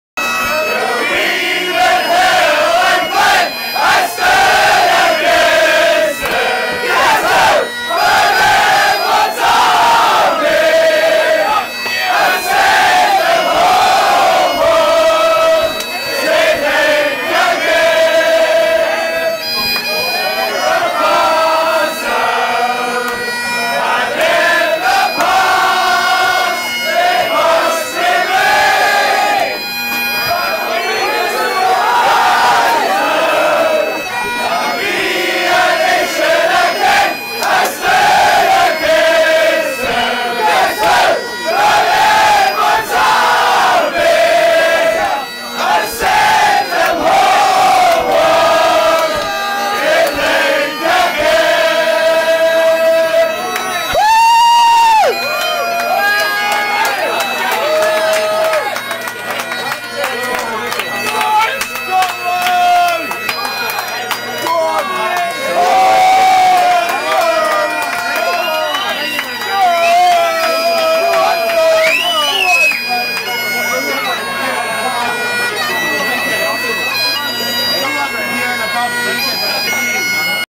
Gary Pipes a Good'un
On Saturday before the heart was ripped from us all in the dying seconds we had this storming moment of optimism when Gary played a blinder to rally the troops